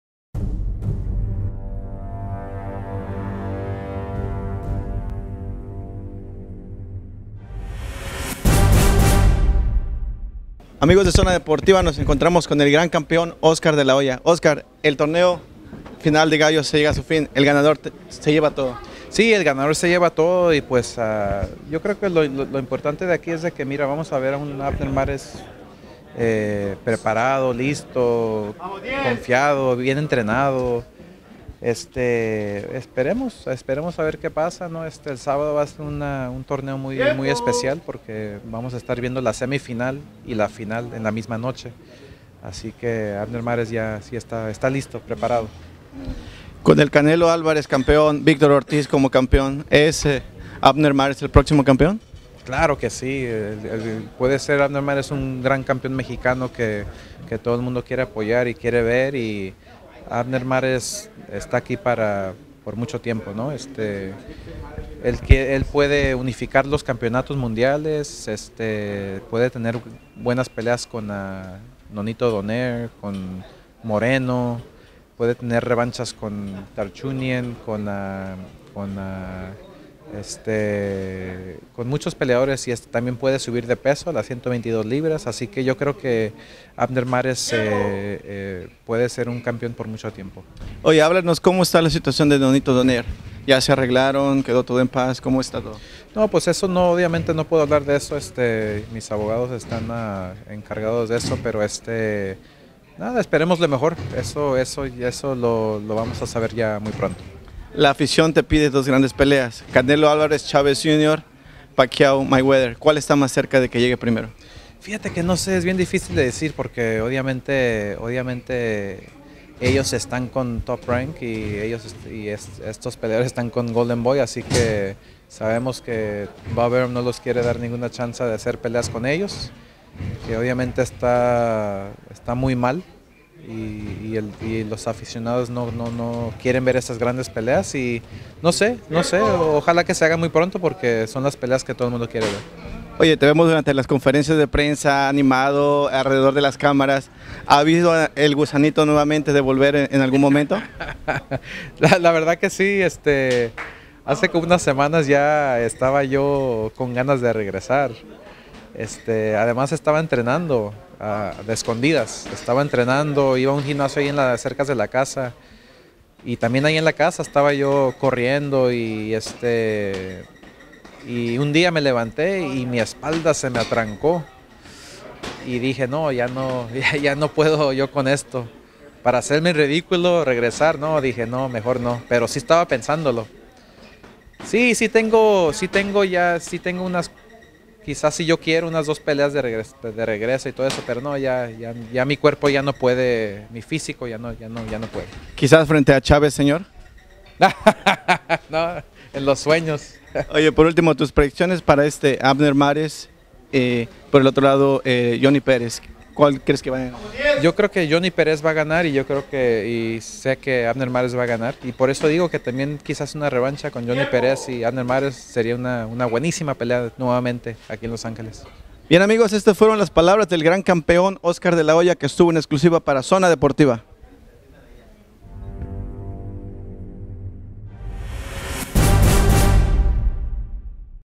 Exclusive Interview With Oscar De La Hoya for Zonadeportivaus.com 4/19/2011
In this interview Oscar De La Hoya talks about Boxing, Manny Pacquiao, Bob Arum and the fight of the century.  Oscar also talks about a possible return to the ring.  This clip is in Spanish.